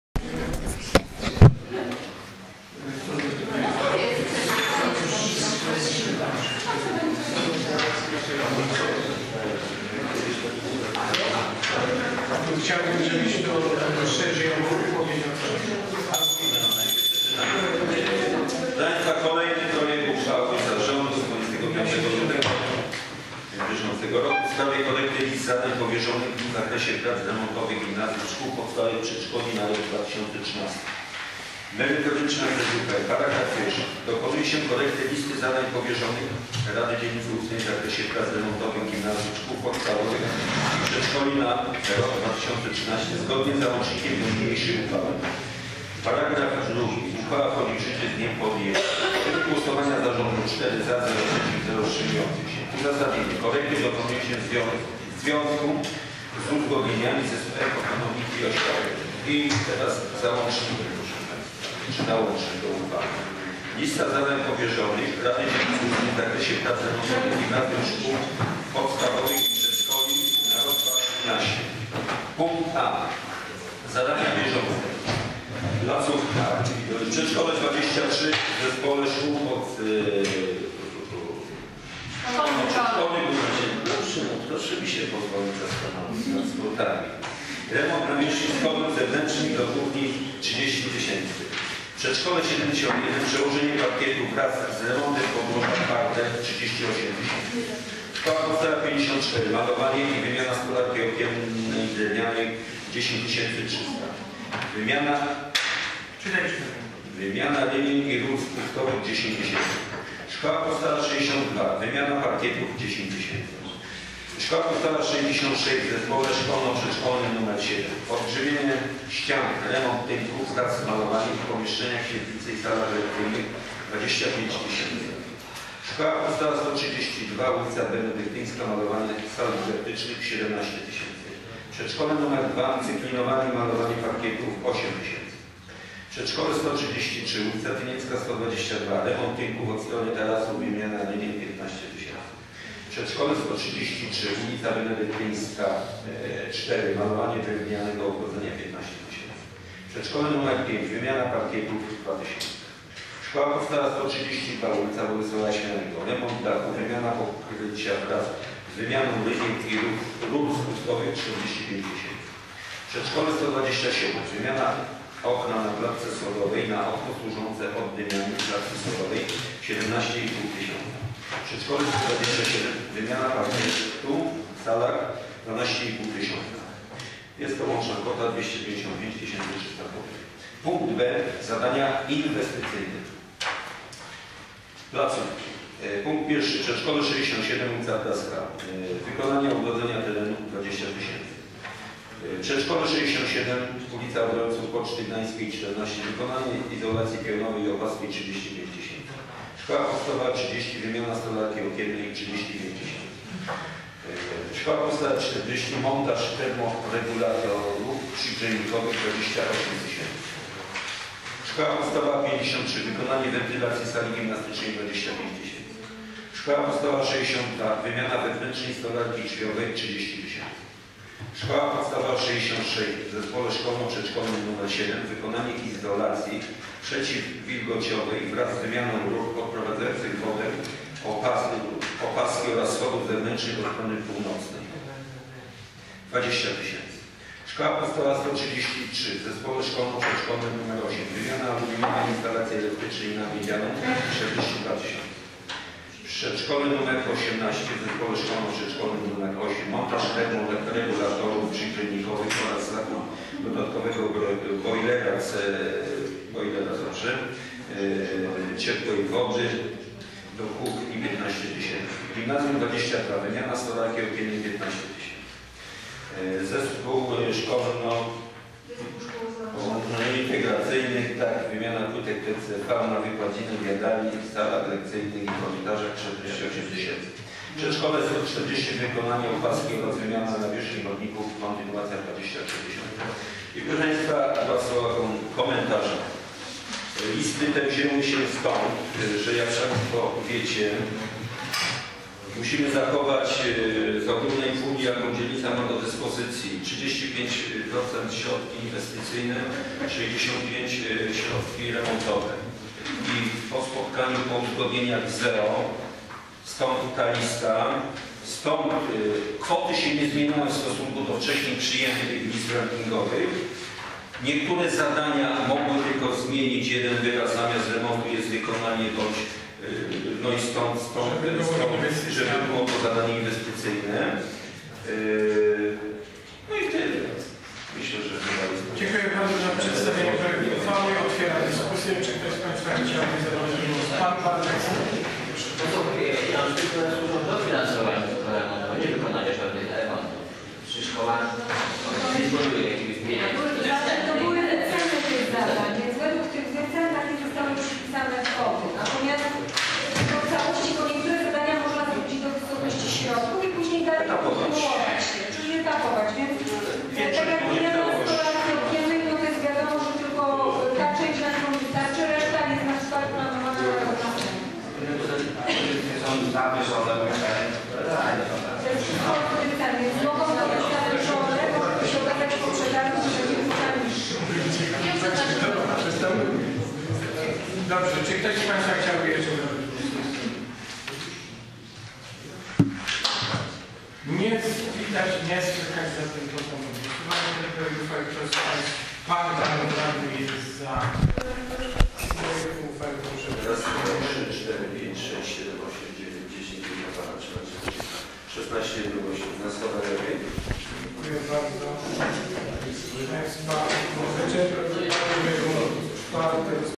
RD_VIII_27-02-2013_15 Uchwała ws. korekty listy zadań w zakresie prac remontowych szkół
Obrady XXXVII sesji Rady Dzielnicy VIII Dębniki z dn. 27 lutego 2013 r. Uchwała ws. korekty listy zadań powierzonych w zakresie prac remontowych gimnazjów, szkół podstawowych i przedszkoli na rok 2013.